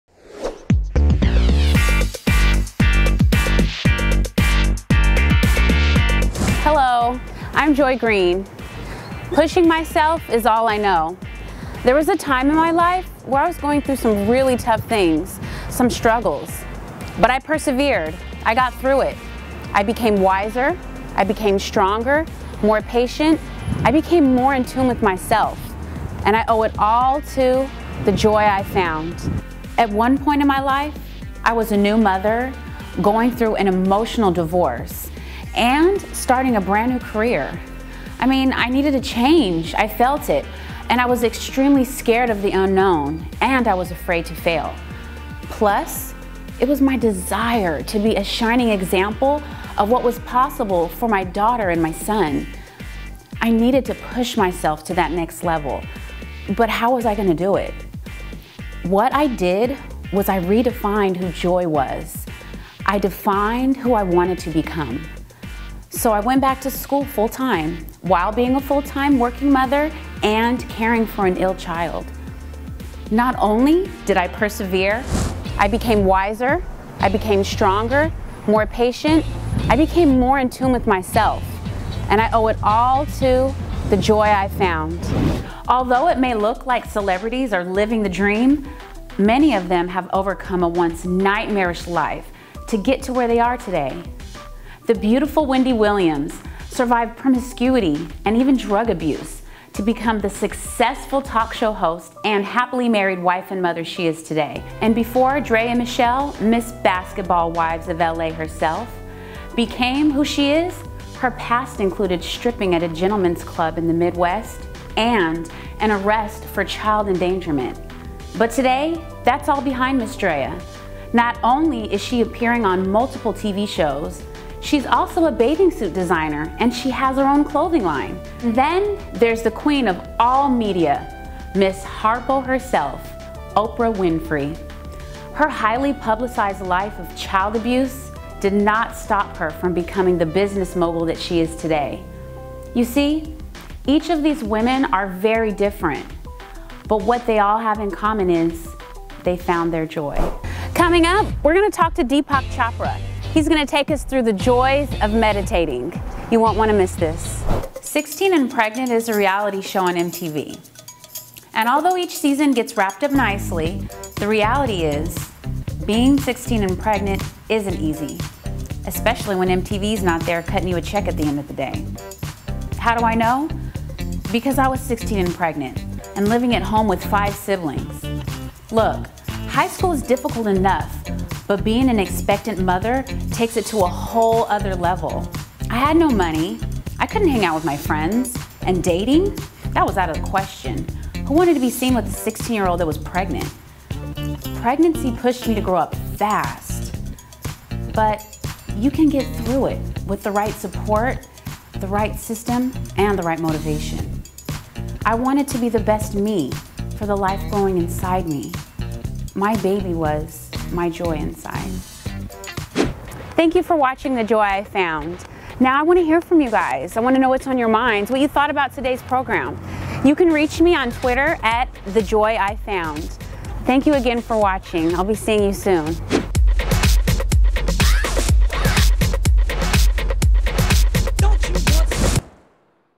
The Joy I've Found
Watch as I share some of my personal trials with divorce and spirituality, with a celebrity spin, and how I pushed through those pains to find joy!
Podcast and book, COMING SOON!!!